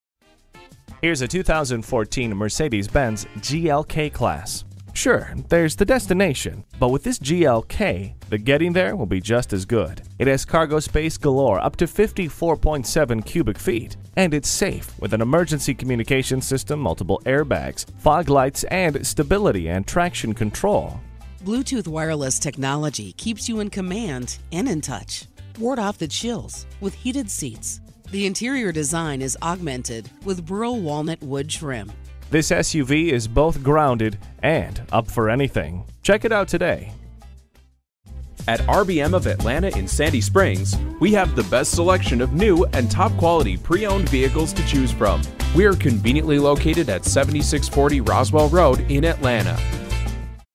2014 Mercedes-Benz GLK-Class Atlanta GA Marietta, GA K7216 - SOLD
Year: 2014
Make: Mercedes-Benz
Model: GLK-Class
Trim: GLK350
Engine: 6 Cyl. 3.5 L
Transmission: Automatic
Color: Polar White
Interior: Sahara Beige/Mocha
Stock #: K7216
The 2014 Mercedes-Benz GLK-Class has an aggressive stance, with gorgeous sculpting on its side profile, a bold oversize grille, and large windows for superb visibility. One year after its makeover, it continues to turn heads everywhere. It is offered in two models, the GLK250 BlueTEC, and the GLK350. The GLK250 BlueTEC comes equipped with a 200 Horsepower, 2.1-liter, inline-4, twin-turbo clean diesel engine, paired with a 7-speed automatic with shift paddles. The GLK350 has the same transmission, but is packing a 302 Horsepower, 3.5-liter, gasoline Direct Injection V-6 engine under the hood for serious performance. While both models offer 4MATIC all-wheel drive, the GLK350 also gives you the option of available Rear-wheel Drive. Both ride proudly atop gorgeous 19-inch, triple 5-spoke wheels. The experience of the GLK interior is simply superb, and will immerse you in comfort and luxury. It is finished with soft-touch materials, and possesses an intuitive and clean instrument panel, Burl Walnut wood trim, and 8-way power-adjustable front seats. Option packages abound for the GLK-Class. There is the available Premium 1 Package, which features items such as a power lift gate, panorama roof, and media interface. Or, the optional Leather, Lighting, or Interior Ambient Lighting Packages might be to your liking as well. The available Multimedia Package is tough to resist with a 7-inch color screen, navigation system, rear view camera, 10GB of space for your music, and Dolby Digital 5.1 sound if you also order the available harman/kardon sound system. Further, it boasts improved Vo

Address:
7640 Roswell Rd